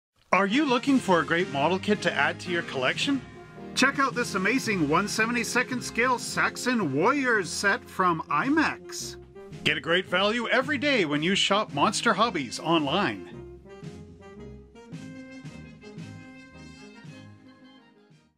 Monster Hobbies - IMEX 1/72nd Scale Saxons Warriors Set Commercial
Imex 1-72 scale Saxons

Are you looking for a great set of a historic Saxon Infantry to spice up your ancient armies, improve your school assignments or for a killer diorama of The Battle of Hastings in 1066?

Then this is the figure set for you!

This set depicts a tribe of Saxon warriors armed with multiple weapons.
It is now available at Monster Hobbies.

This is an unfinished plastic model kit
Shrink wrapped
Paint and cement not included
50 figures in assorted poses
Hard but pose-able plastic
Painting chart included on back of box
Special Features:

Thirteen sets of fifty individual figures
Model kit is easy to assemble and paint
Plastic is pose-able to change the look of the figures

Overall impressions:

The 13 poses display plenty of action and demonstrate good realistic movement.
None of the figures include any that are on the march, nor any that would suggest the famous Saxon shield wall tactic, but a skilled hobbyist with some warm water can re-position the pose-able plastic to have their shield arms moved to provide suitable wall figures.

Since swords were expensive for Saxons, very few of these figures carry them. To be more realistic to the time period, half the figures in the set carry spears or javelins and axes are also plentiful. Although archers are included in this set, The Saxons never used them as a major part of their battle strategy. The bows here are about the height of the archer, which is thought to have been the actual size. A knife was an essential implement, although no figure is actually using one. The round shields are a reasonable size and their handles are situated in the middle of the shield. Most of the figures carry a shield. All the weapons are good and appear in about the right proportions.

This Imex set is certainly the best Saxon warriors yet produced.

Easy construction suitable to beginner modelers.

General Information :

Figures and kit parts come unpainted.
Paint and cement are not included.
Plastic is non-toxic.

Cautions :

This figure set is designed for ages 6+.
It contains small parts which are not suitable for children under 6 years of age.
It is the consumer's responsibility to keep this product away from small children.